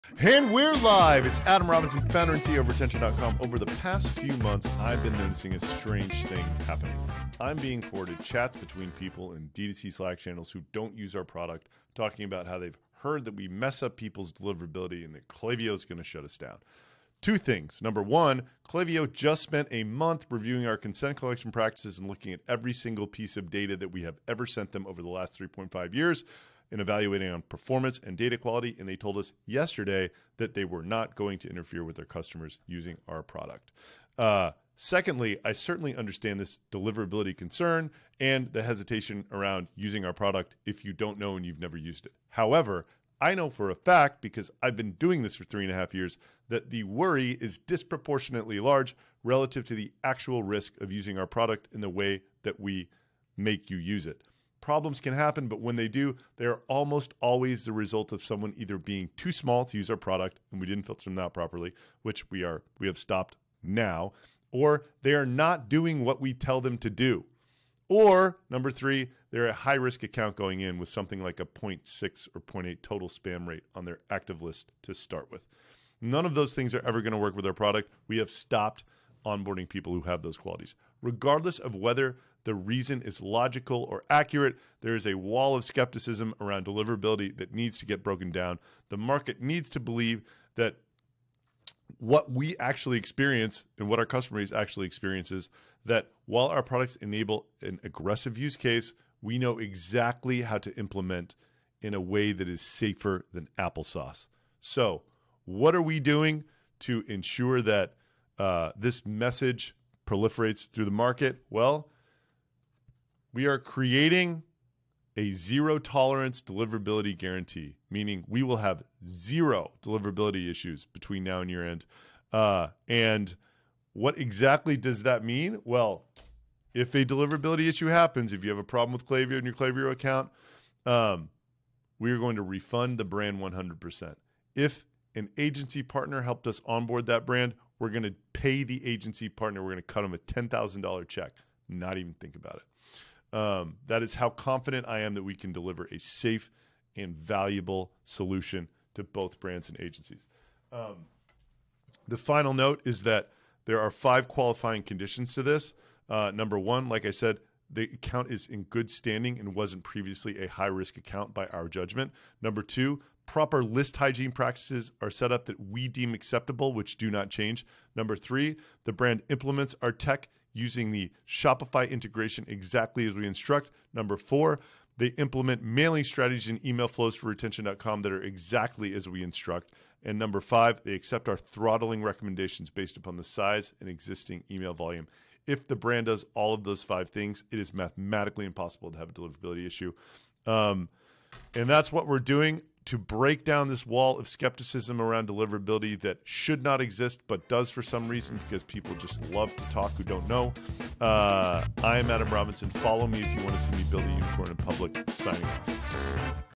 Zero Tolerance Deliverability Guarantee
🛑There’s some rumors roaming around that I want to put to rest 🛑

Some people (who by the way, don’t even use our product) “heard'' that Retention’s messing up peoples deliverability and Klaviyo is going to shut us down. 💀

Well, that just isn’t true. In fact, Klaviyo just spent a month reviewing our consent collection practices and looking at every single piece of data that we’ve ever sent them. They have reassured us that they will NOT be interfering with customers who use our product.

Of course, I 100% understand the skepticism around using a product like ours and the worry that someone could have around deliverability rates being affected.

So, in this episode, I’m going to break down our process and The Zero Tolerance Deliverability Guarantee that we’re putting in place to ensure our customers stay protected!